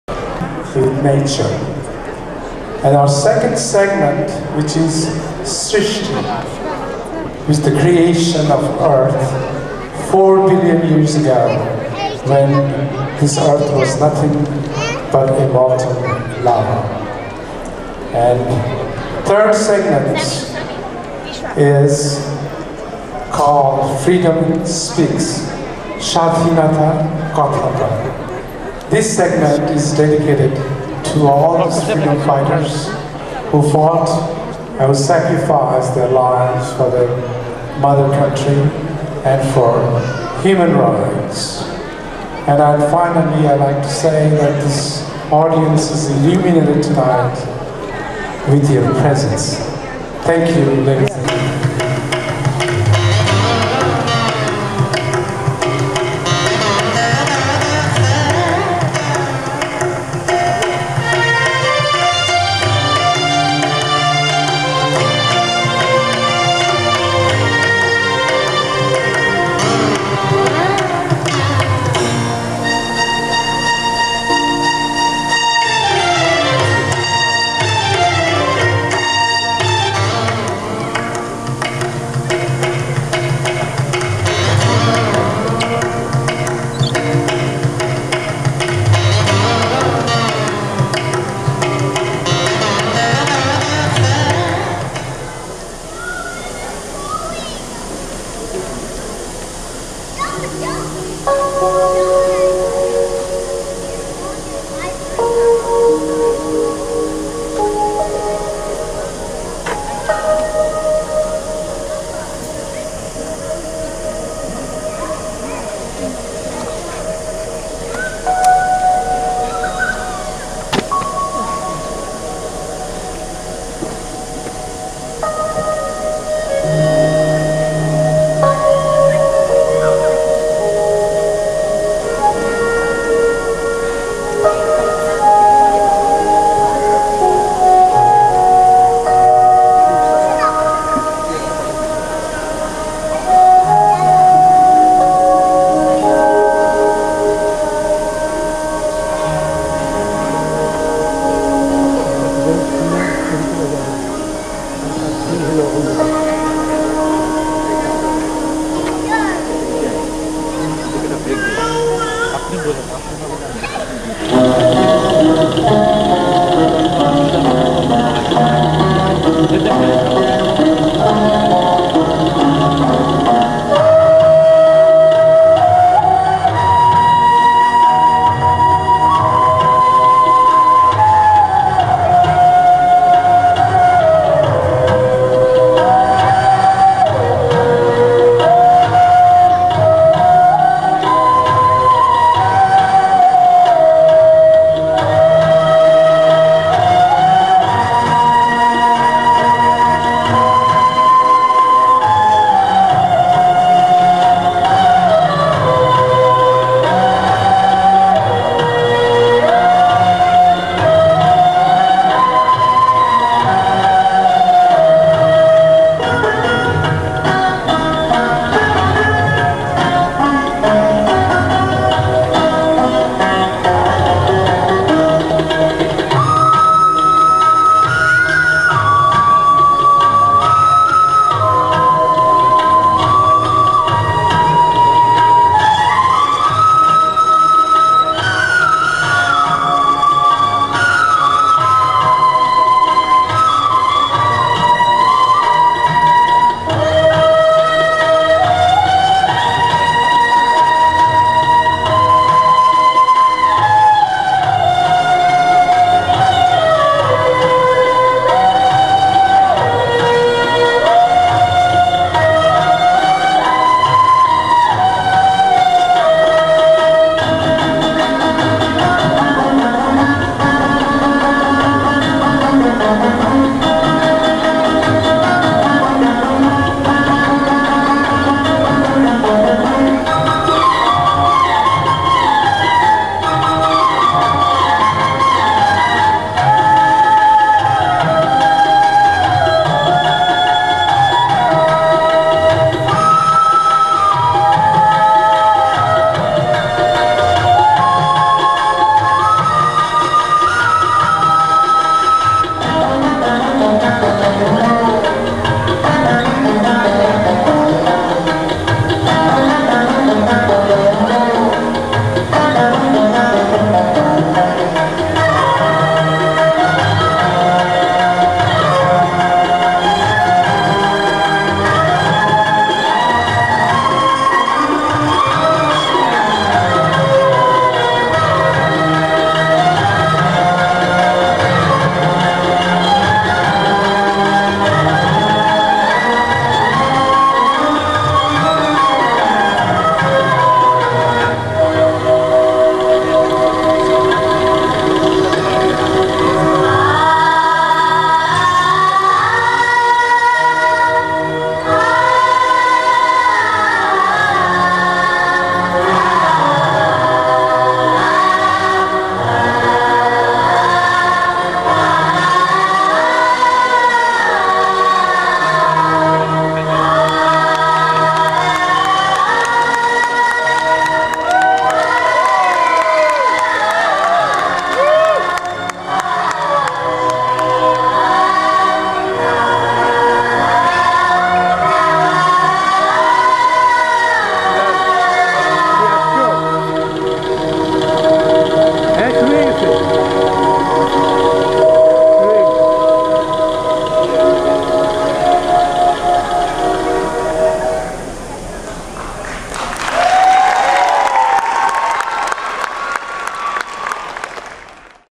FOBANA 2009 - Group dance by BAH (Clip14)
Opening night of FOBANA 2009 at George Brown Convention Center, Houston, Texas on July 2, 2009. Group dance by BAH. (NH)